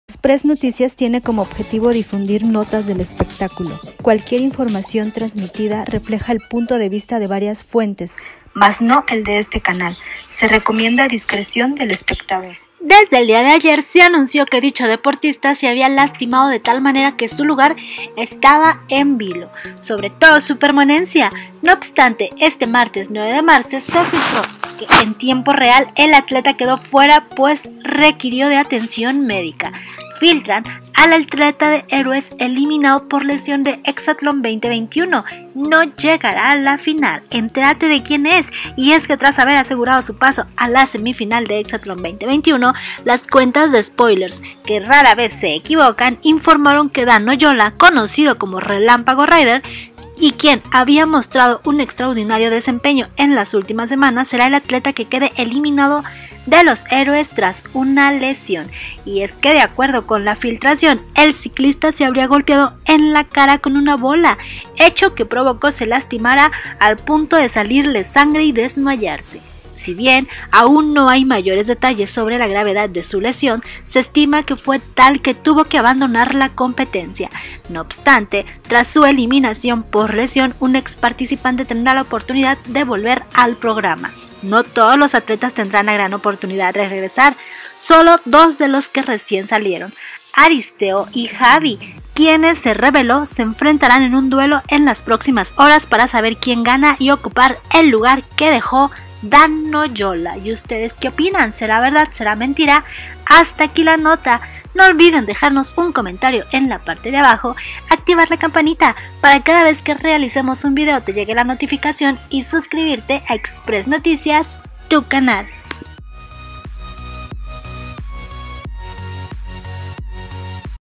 FILTRAN al atleta de Héroes que no llegará a la final, es ELIMINADO por lesión
Malas noticias para los fans azules. Uno de los atletas que ganó su pase a la semifinal de Exatlón 2021 quedará fuera de la competencia tras sufrir dolora lesión, checa de quién se trata.UNETE A NUESTRO CANAL HERMANO